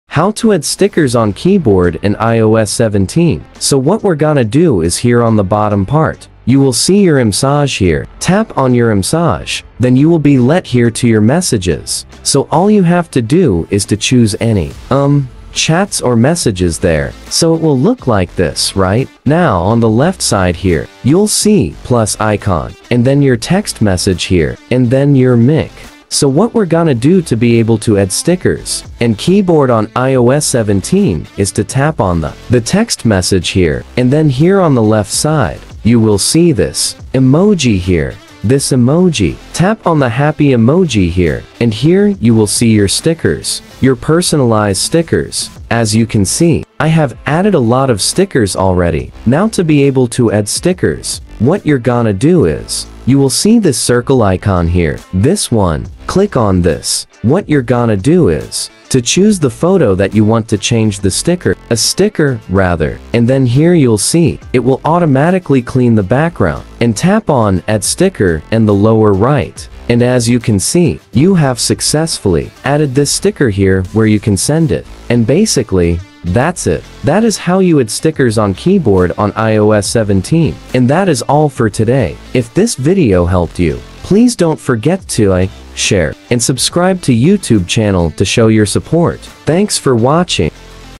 How To Add Stickers To Keyboard On iOS 17 2023 [easy]
In this quick and informative tutorial, we'll guide you through the simple process of adding stickers to your keyboard on iOS 17, the latest iOS version for iPhone. Whether you want to express yourself with fun emojis or add a touch of creativity to your messages, stickers can make your conversations more engaging.

We'll show you step by step how to access the sticker collection, add your favorite ones, and use them while chatting with your friends and family. It's an easy way to add a little flair to your messages!

Join us on this journey to enhance your texting experience.